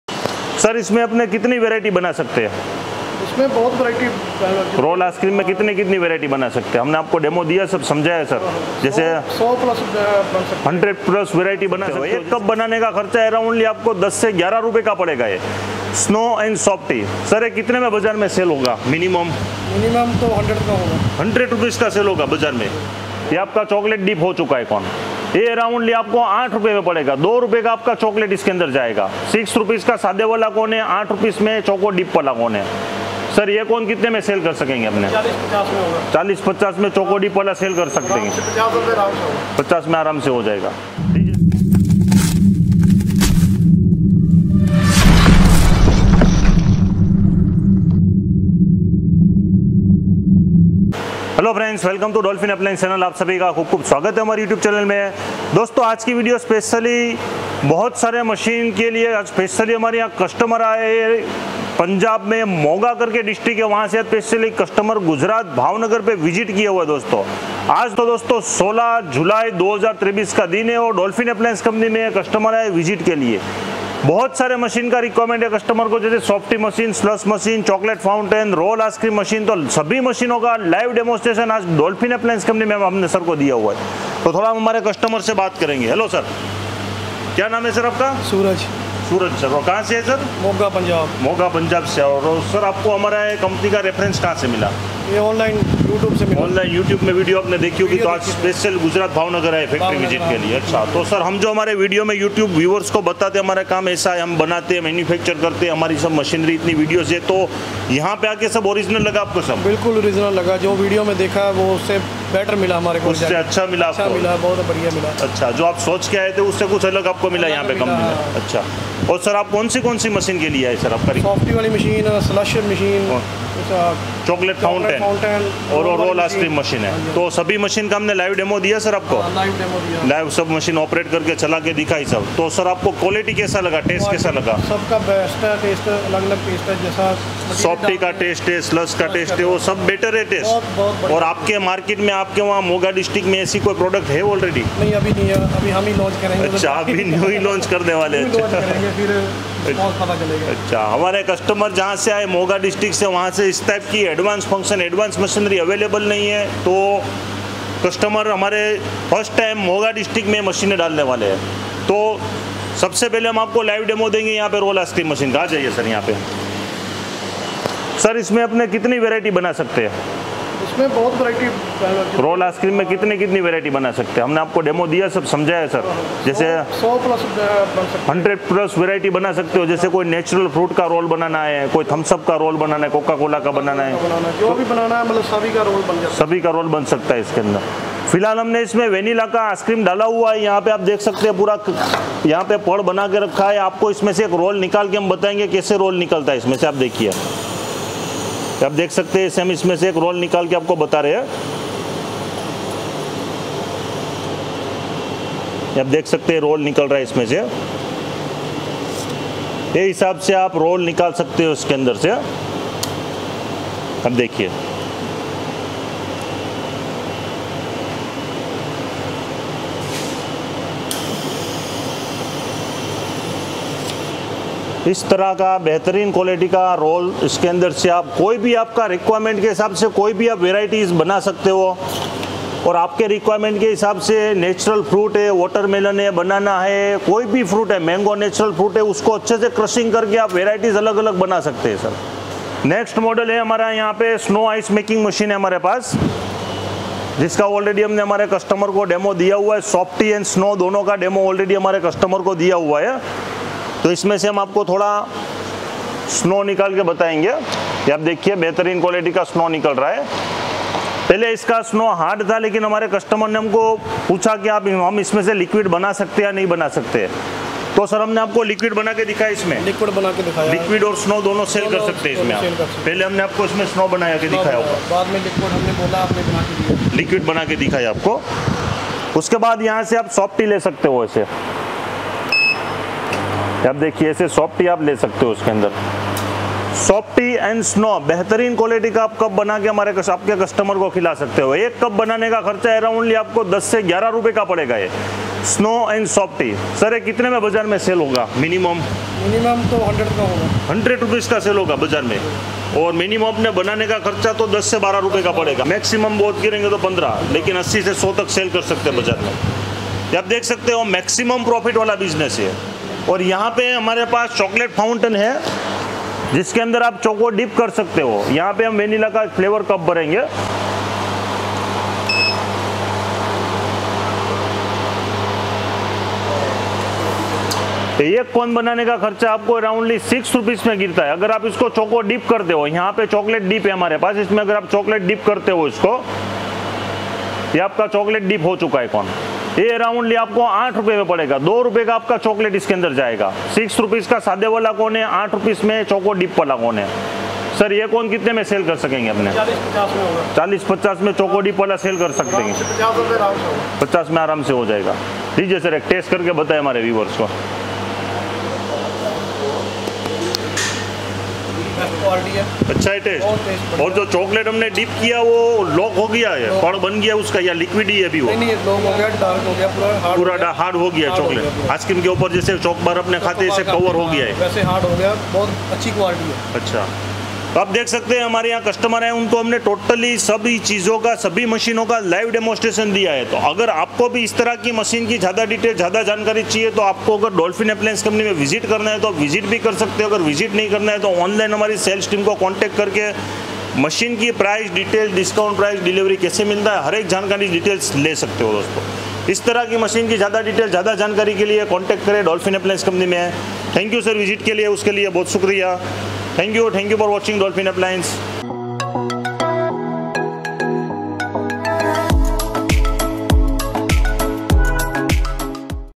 २०२३ के ४ ऐसे बिज़नेस जिससे आप कमा सकते हे रोजाना र १५००० तक | Ice Cream Machine | Slush Machine
dolphin appliances
alka cinema road, \bhavnagar- 364001, gujarat